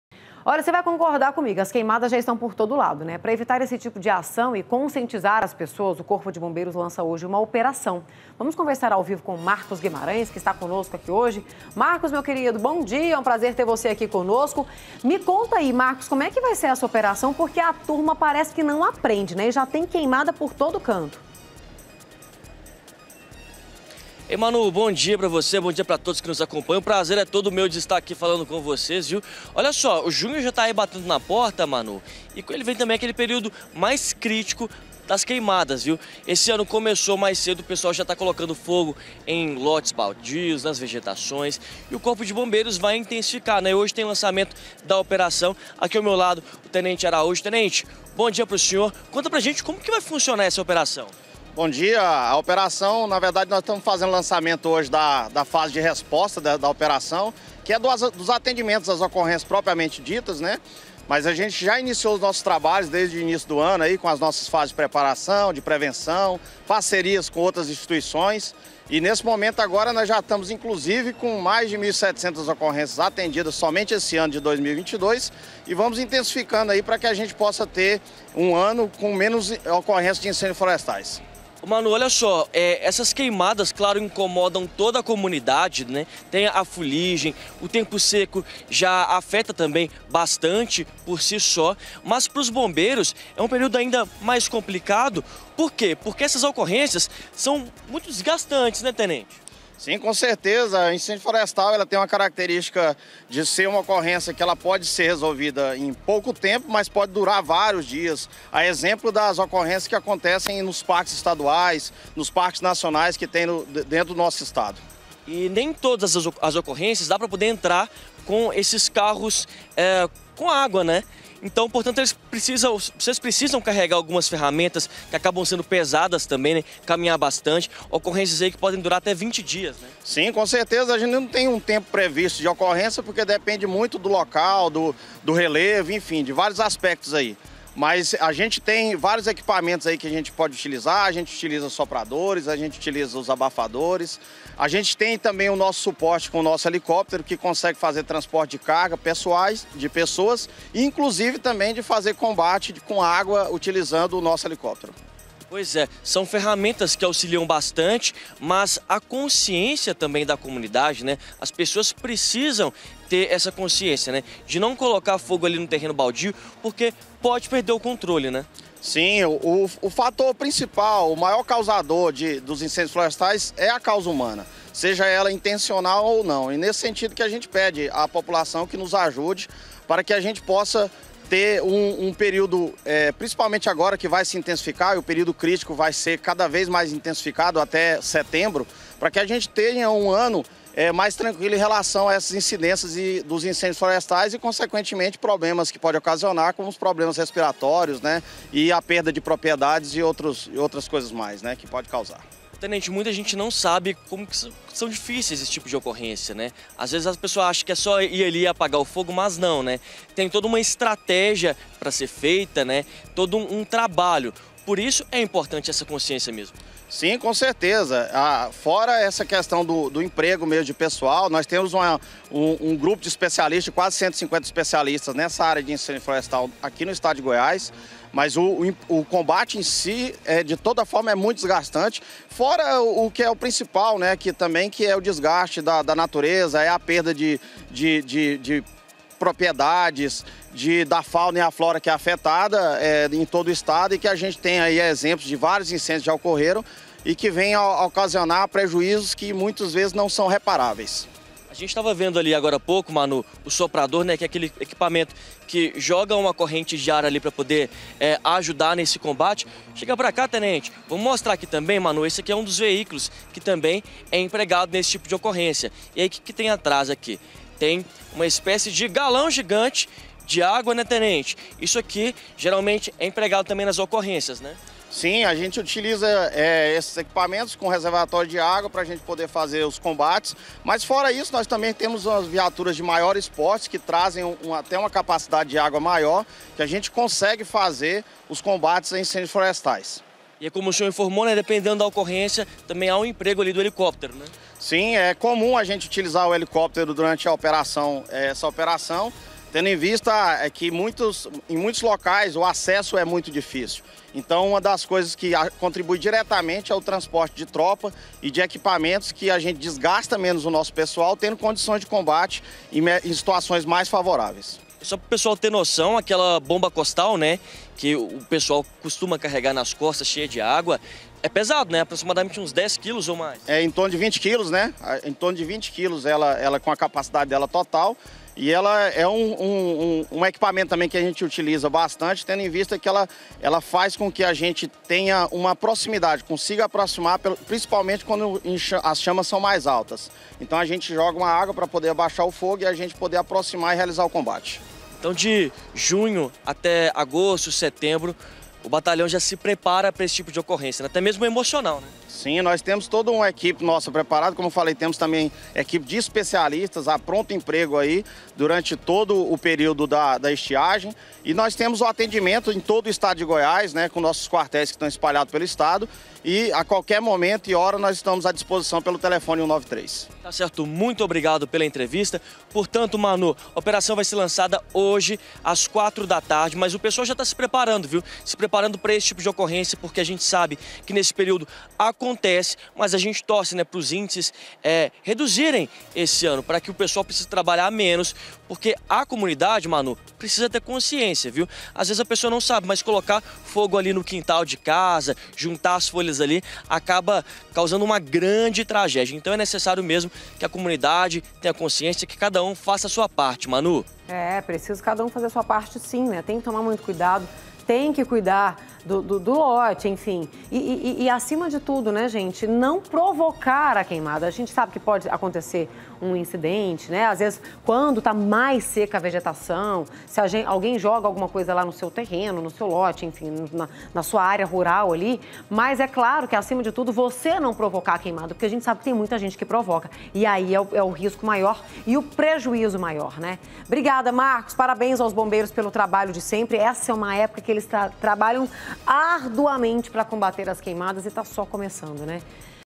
OPERAÇÃO CONTRA QUEIMADAS: CORPO DE BOMBEIROS ALERTA PARA OS RISCOS
VOCE VAI CONCORDAR COMIGO, AS QUEIMADAS JÁ ESTÃO POR TODO LADO. PARA EVITAR ESSE TIPO DE AÇÃO E CONSCIENTIZAR AS PESSOAS O CORPO DE BMBEIROS LANÇA HOJE UMA OPERAÇÃO.